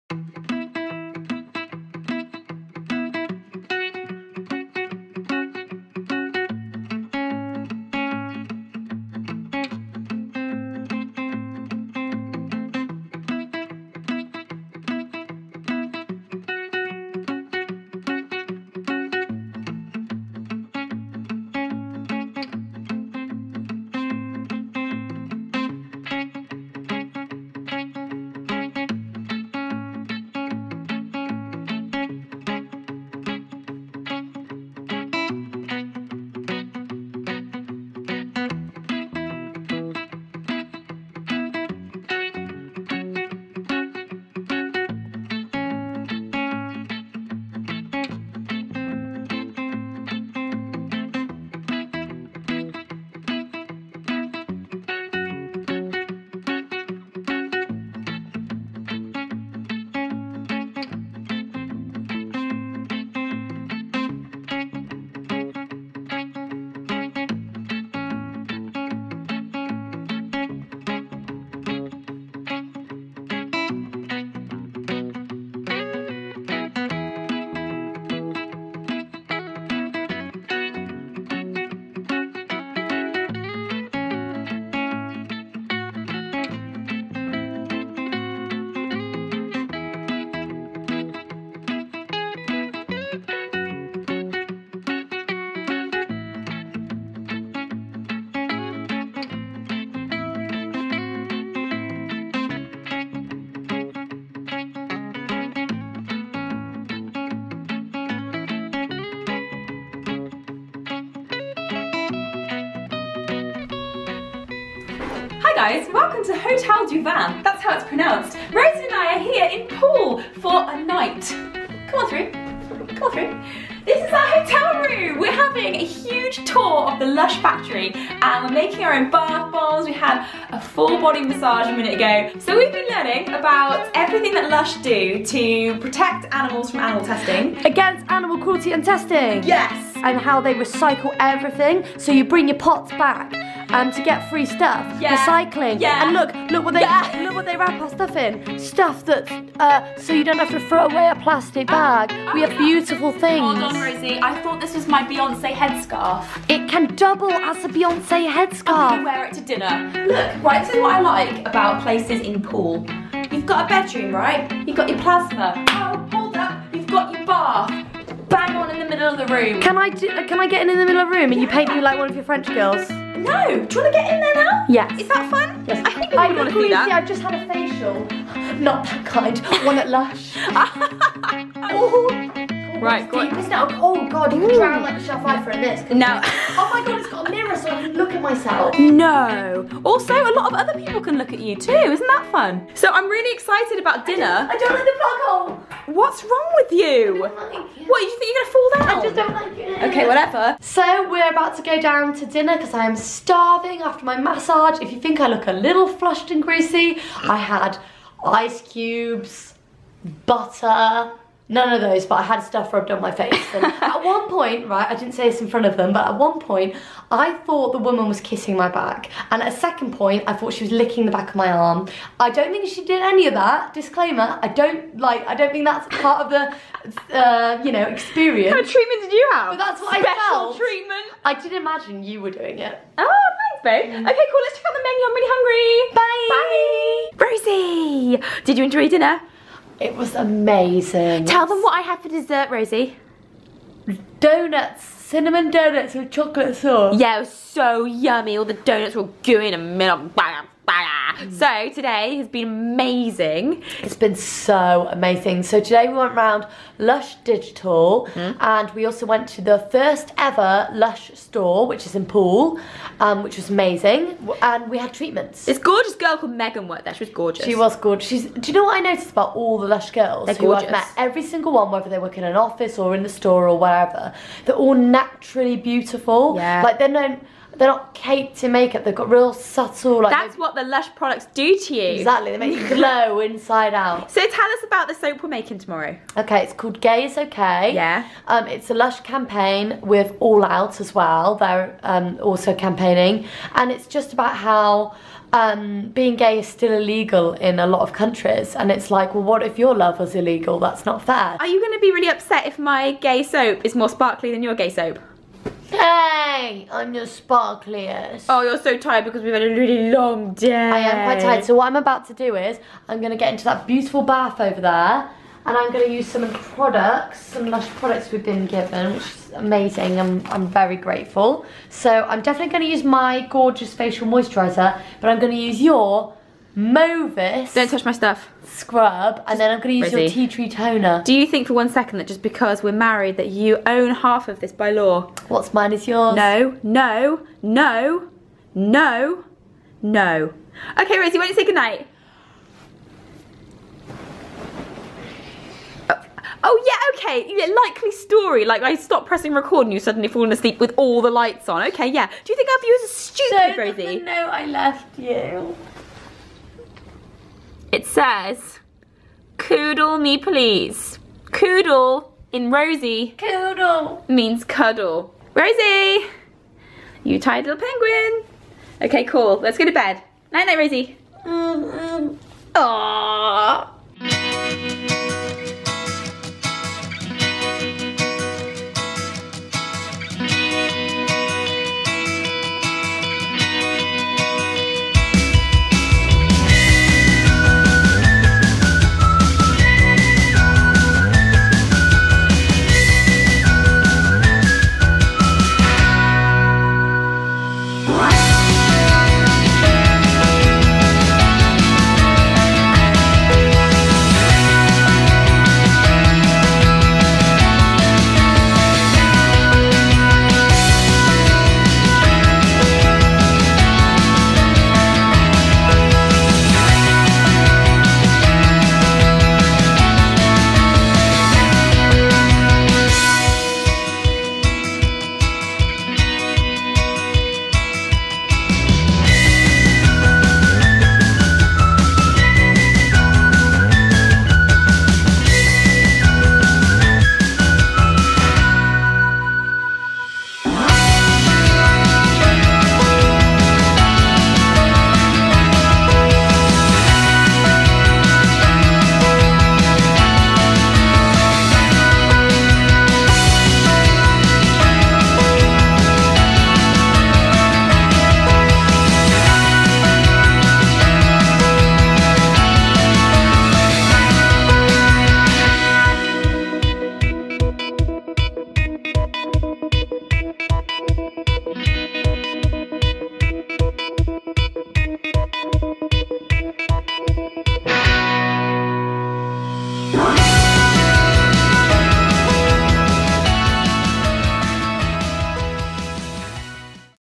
VlOG | MAKING LOVE... SOAP!
Woot WOOOO just look at that camera quality! You don't see images looking as fine as that everyday! It was an absolute pleasure vlogging our trip with that new badboy! Anyways so behind this video is a super serial message. I know we've all been celebrating after the USA came through with the new laws on gay marriage and then we got to get all proud at pride right after that. But while all that awesomeness is going on we cannot turn a blind eye or forget that in 75 countries homosexuality is a CRIME. However Lush have come up with the Love Soap and you can do your part to help! You can buy a soap in your store or online and the profits will be put into Lush's 'Love Fund' and then given to LGBTQAI groups around the globe. If you have no pennies to buy pretty things with then you can STILL help by going into your local store and taking a SELFIE with the soap and using the hashtag GAYisOK to help the campaign reach more people who can buy the soap and donate money. Can't get to a soap to take a selfie with? Just use the hashtag! Every little helps!!

Rose and I had soooo much fun looking around Lush and hearing all about how they recycle and only use ethically bought produce and use plenty of natural ingredients and are against animal cruelty and just generally awesome!! In fact, I've been inspired to write lots and lots of blog posts all about it!

Okies well I could write in here forever because I reallllllllyyyyy love writing things down, but I should probably go and write a list or a blog post or something. Ok byeeeeee!! You can also find us here!

P.S  Whacky loves Cuddly forever